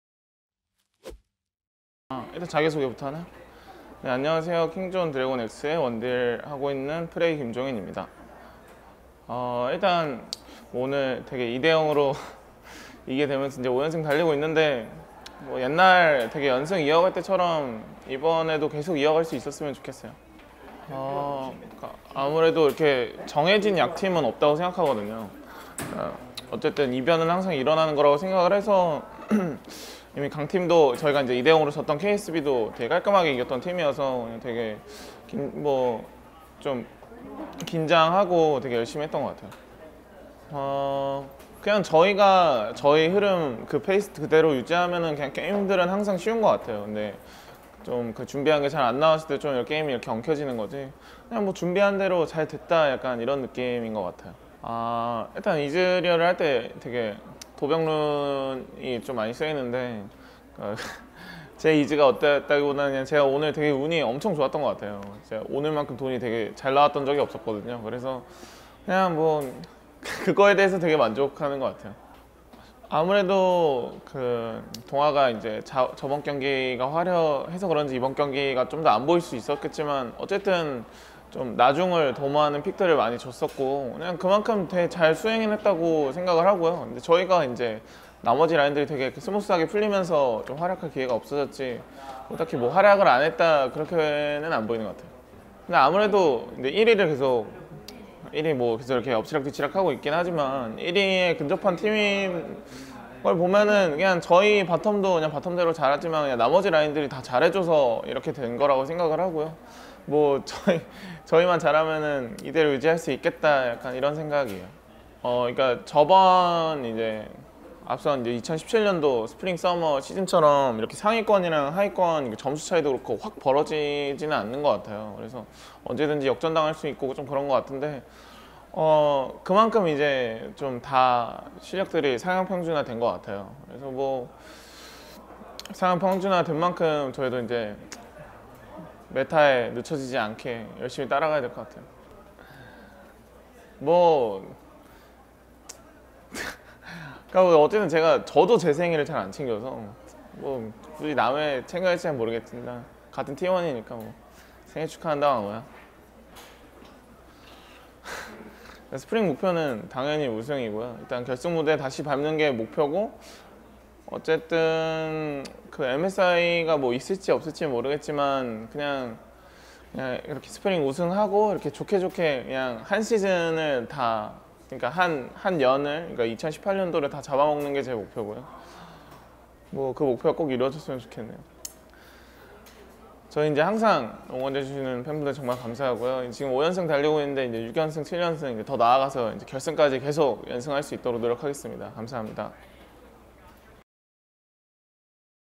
'프레이' 김종인 "2018시즌 모두 잡고 싶다"
20180204 '킹존' 프레이 롤챔스 인터뷰